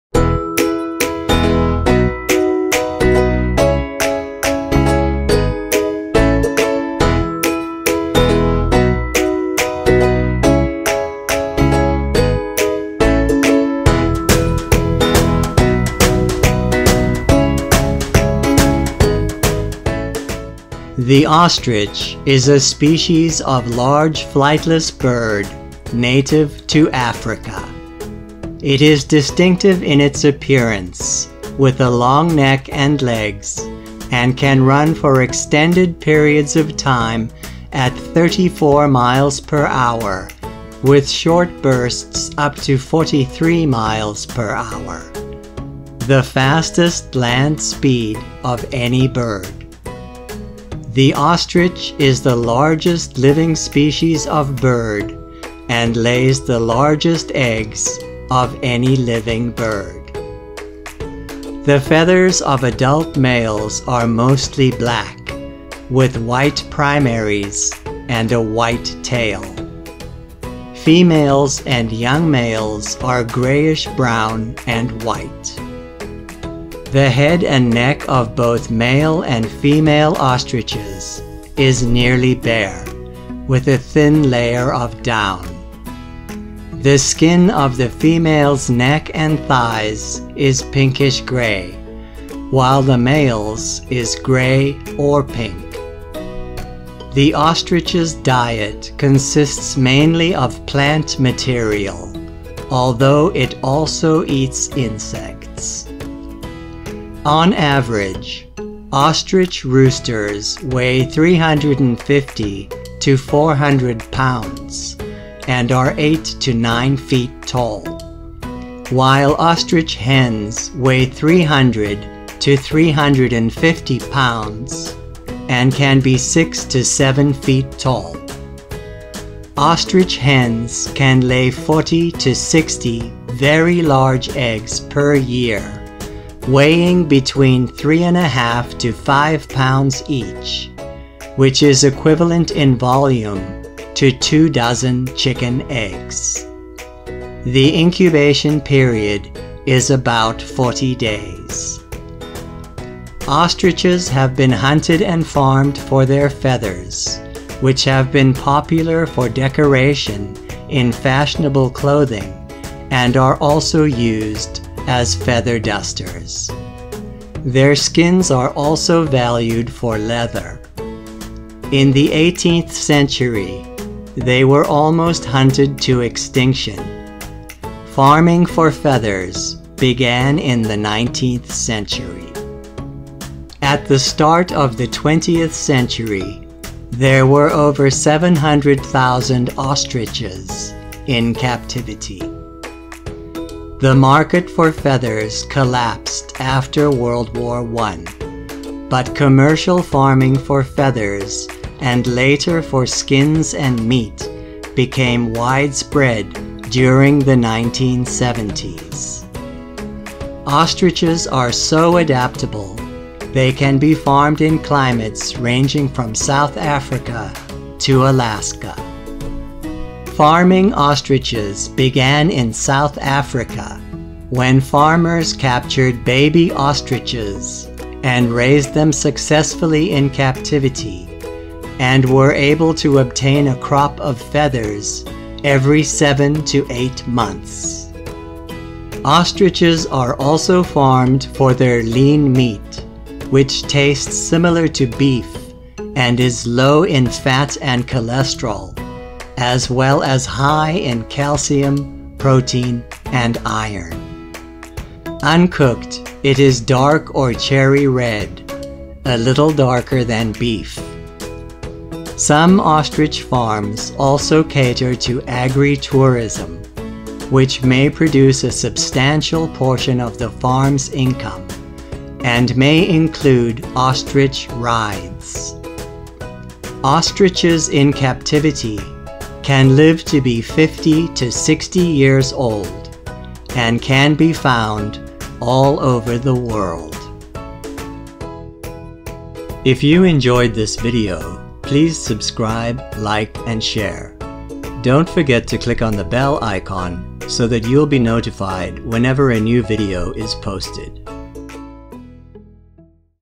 Ostrich | Huge Birds Lean Meat
Ostrich farming began in South Africa when farmers captured baby ostriches and raised them successfully in captivity and were able to obtain a crop of feathers every seven to eight months.